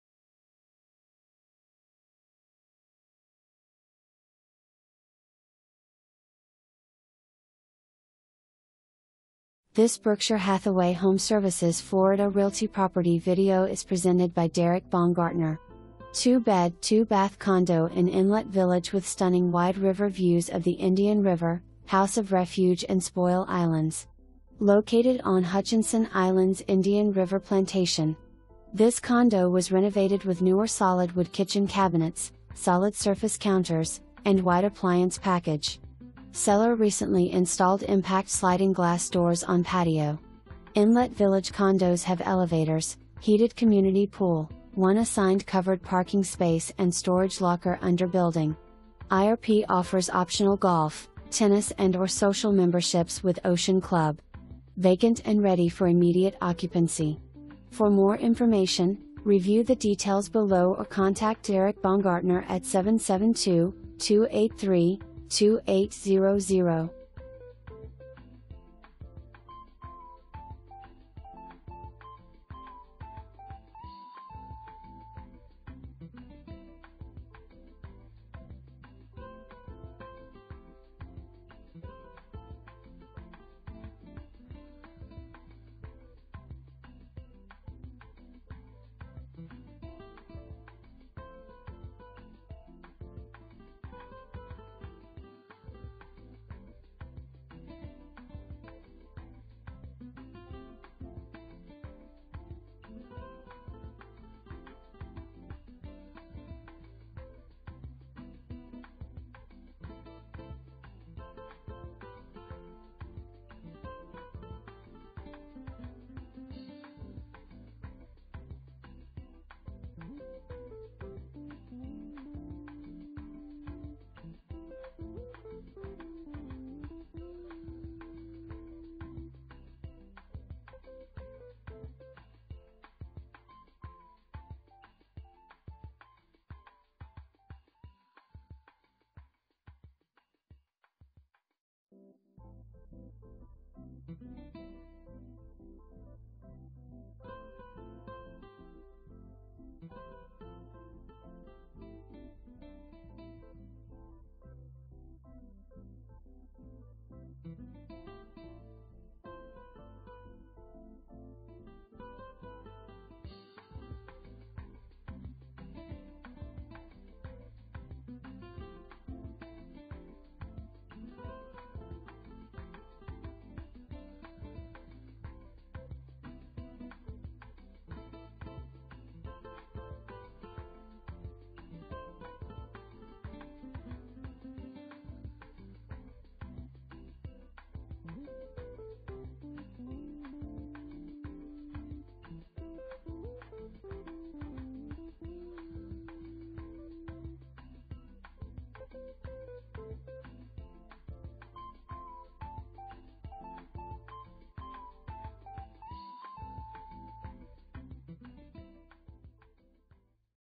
Berkshire Hathaway HomeServices Florida Realty - 20 NE Plantation Road
2Bed 2Bath condo in Inlet Village with stunning wide river views of the Indian River, House of Refuge & Spoil Islands. Located on Hutchinson Islands Indian River Plantation. This condo was renovated with Newer Solid wood Kitchen Cabinets, Solid Surface Counters, & white appliance package. Seller recently installed Impact Sliding glass Doors on Patio. Inlet Village Condos have Elevators, heated community Pool, 1 assigned covered parking space & storage locker under building. IRP offers Optional Golf, Tennis & or social memberships with Ocean Club. Vacant & ready for immediate occupancy.

Derik Baumgartner
Berkshire Hathaway HomeServices Florida Realty